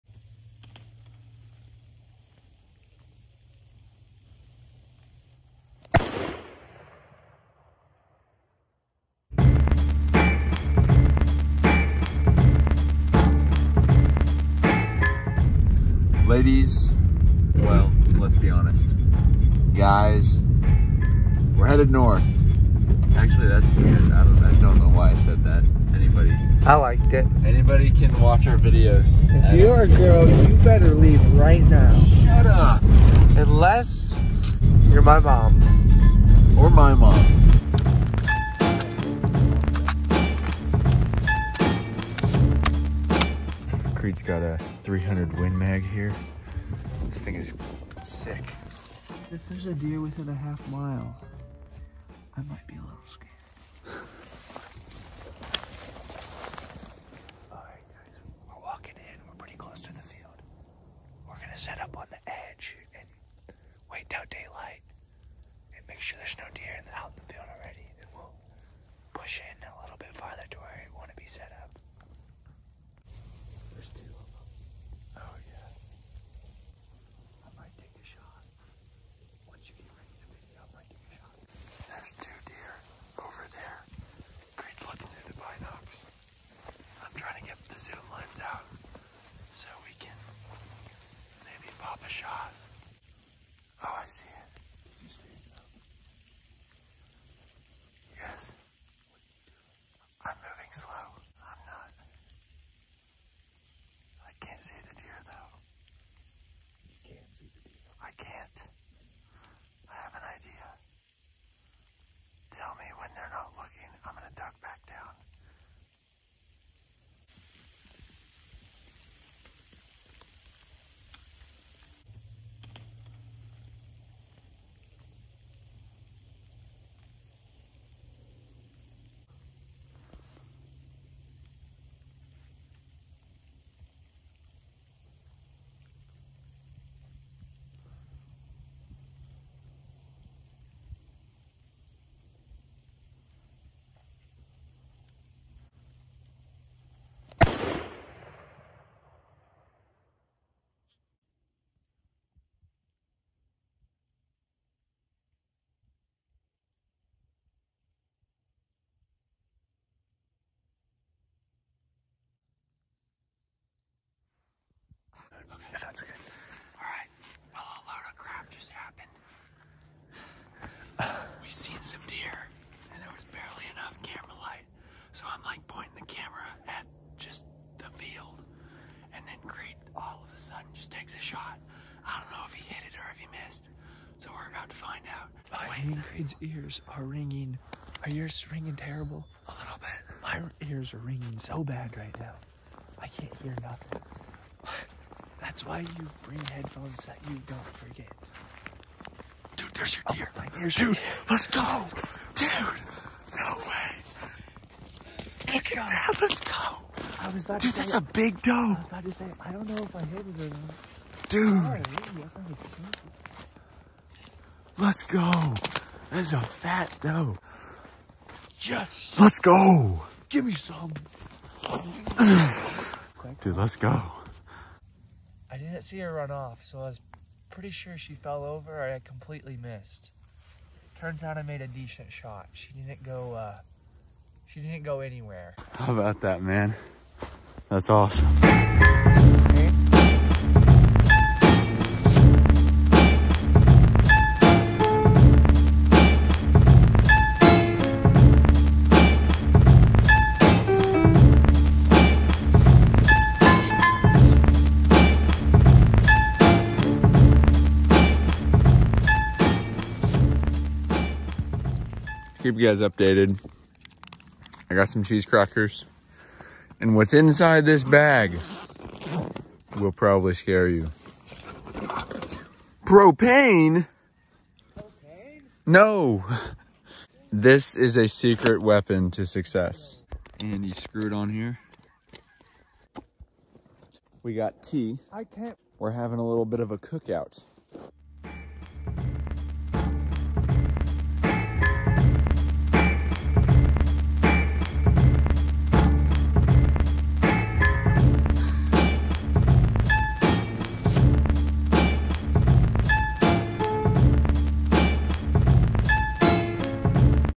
Deer Hunting in a BLIZZARD!!!
Wild snow storm brings out a bunch of nice deer!